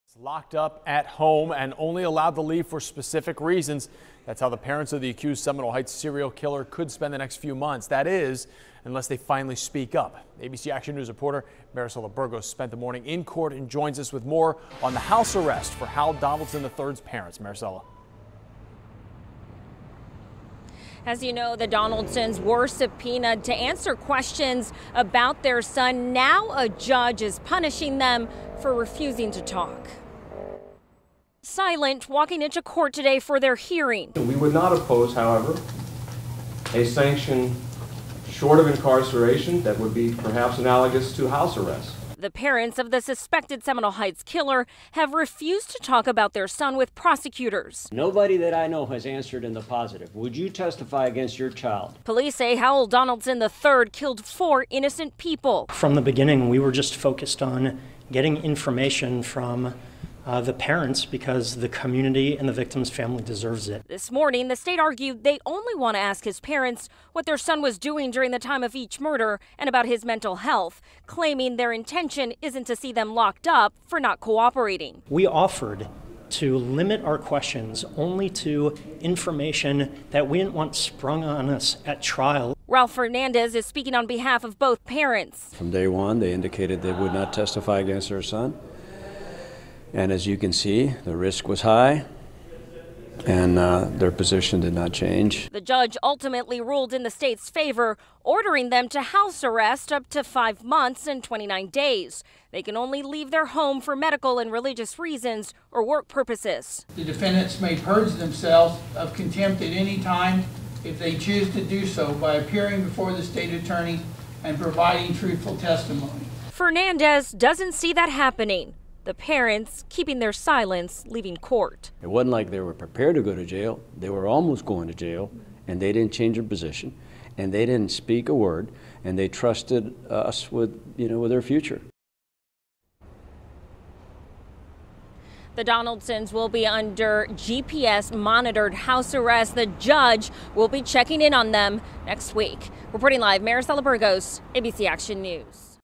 Parents of accused Seminole Heights killer held in contempt of court, ordered to house arrest
The parents of the accused Seminole Heights killer are being held in contempt of court for not answering prosecutor's questions. They have been placed on house arrest with the exception of church, medical issues and employment schedules.